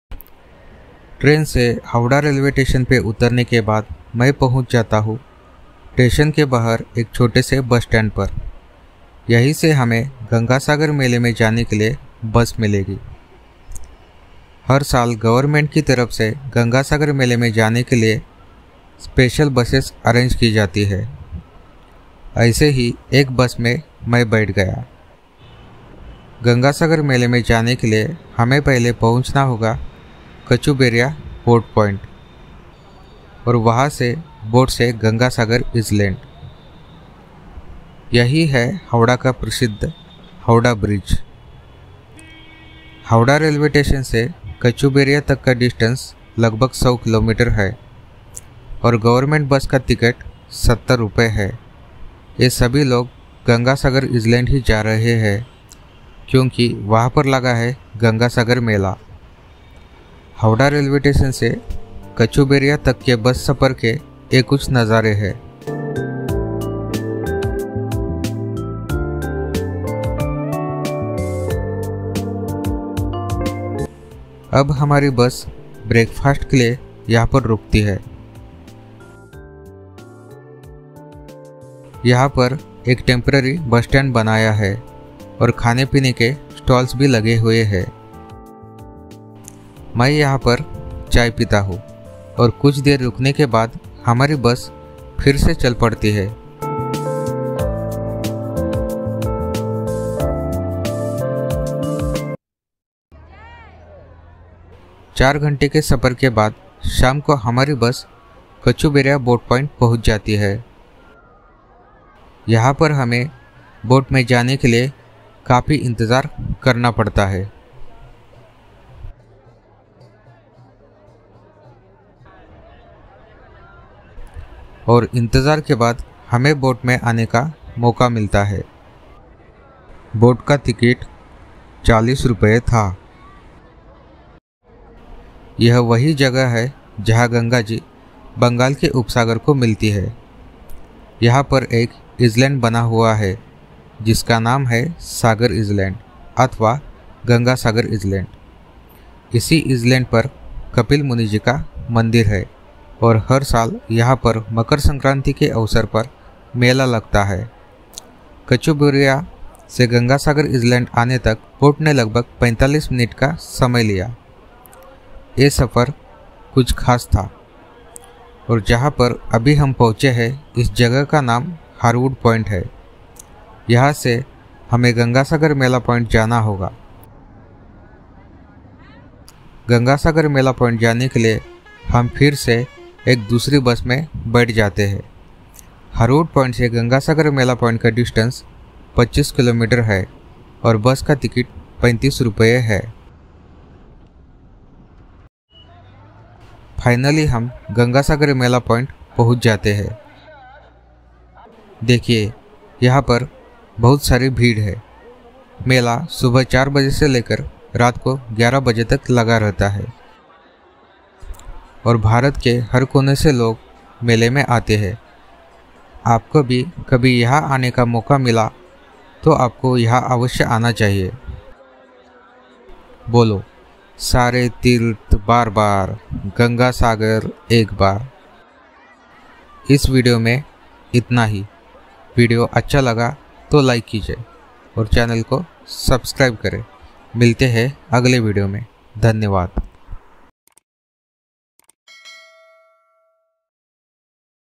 गंगासागर मेला व्लॉग | How to reach Gangasagar Island from Howrah or Kolkata | Kachuberia Boat Point
Every year, on the occasion of Makar Sankranti Gangasagar Mela is celebrated at Gangasagar Island which is near about 100 km away from Howrah or Kolkata city of the West Bengal.
Mela point is at Gangasagar which is small island in  river Ganges delta.  During Mela time, millions of devotees offer prayer in Kapil Muni Temple.
Every year this fair is well organized by West Bengal State Government. Special buses are arranged from Howrah railway station to Sagar Island & also from Kolkata city. Tents & toilets are also arranged. Sagar island is home for thousands of people, they are engaged in farming & fishery. This fair is celebrated for one week, Makar Sankranti day is main day of the fair.
This fair is the second largest congregation of mankind after the triennial ritual bathing of Kumbha Mela.